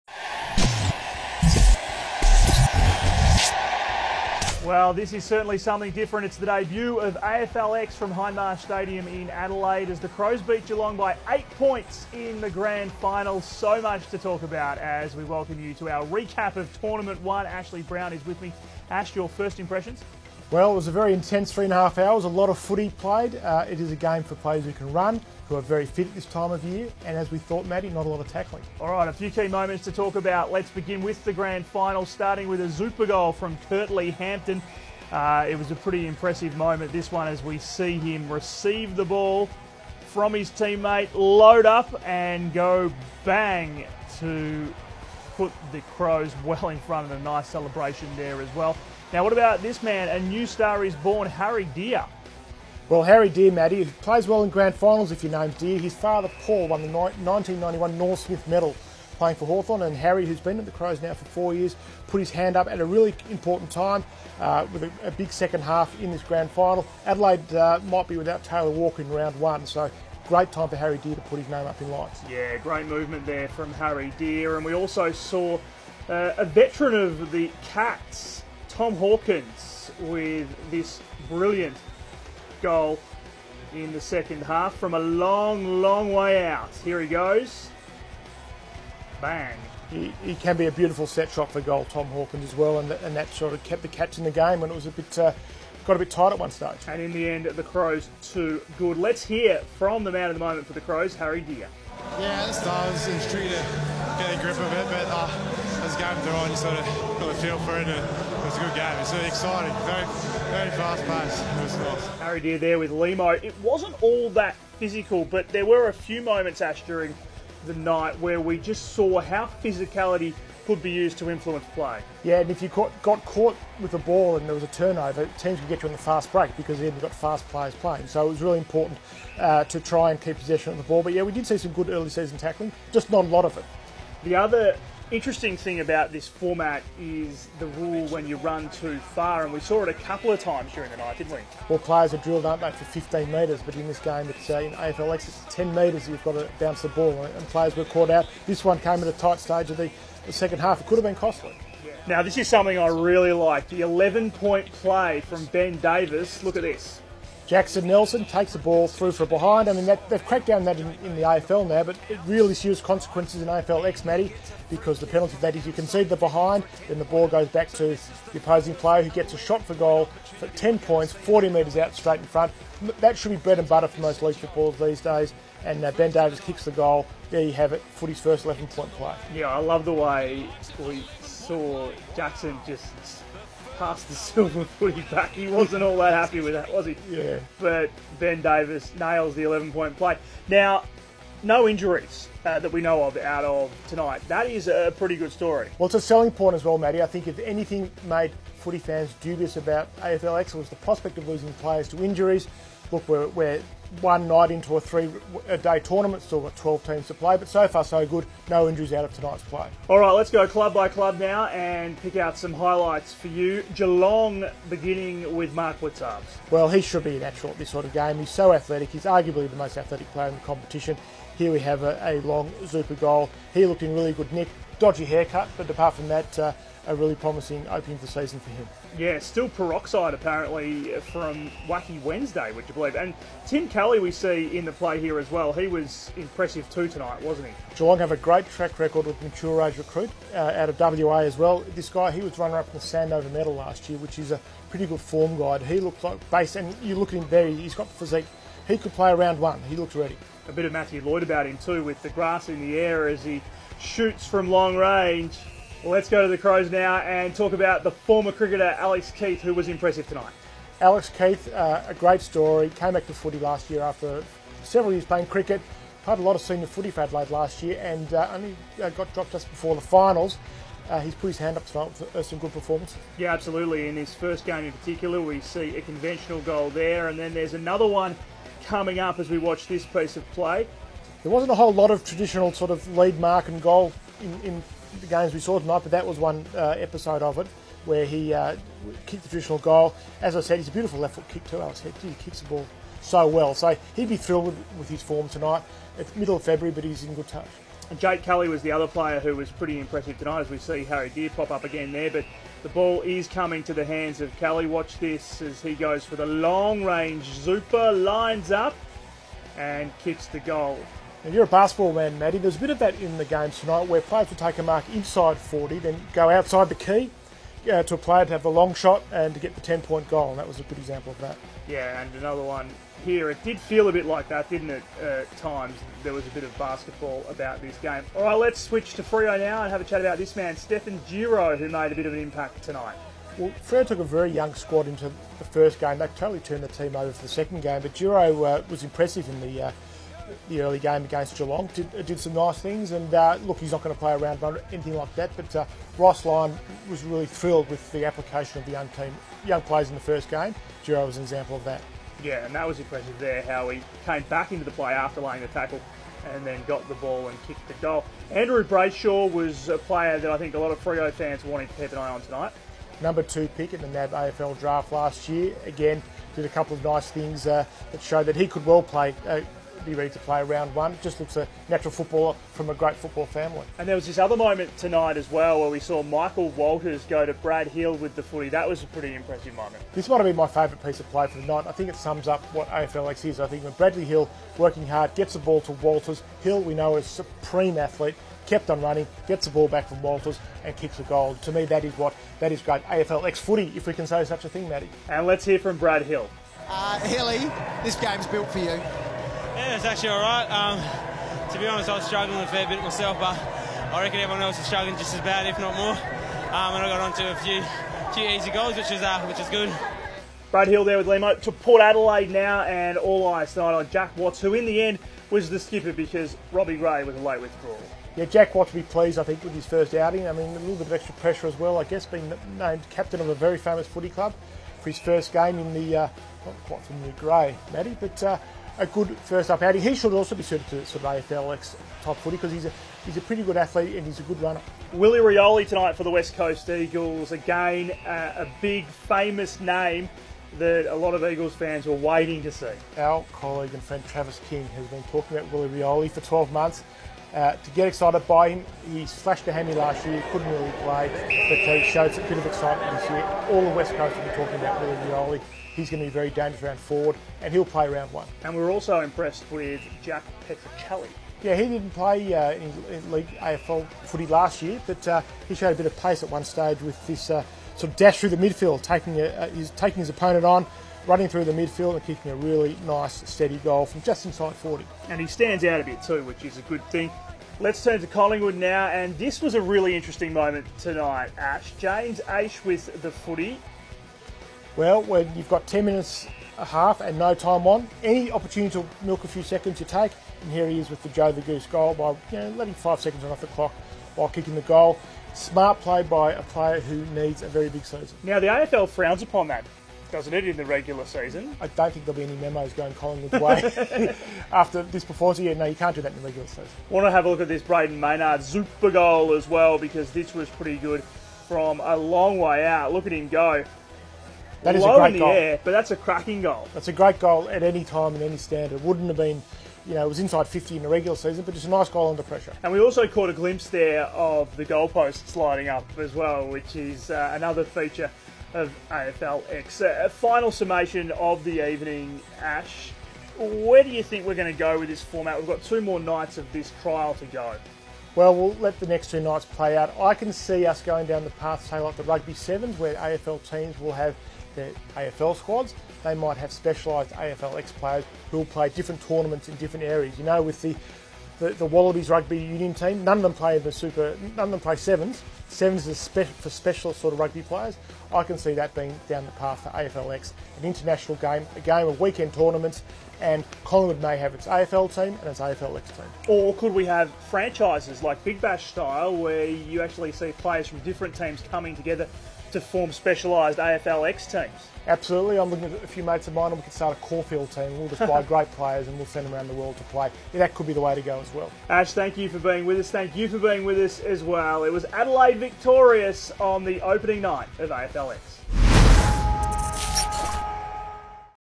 Adelaide's historic night of AFLX
The main moments from Adelaide with Matt Thompson and Ashley Browne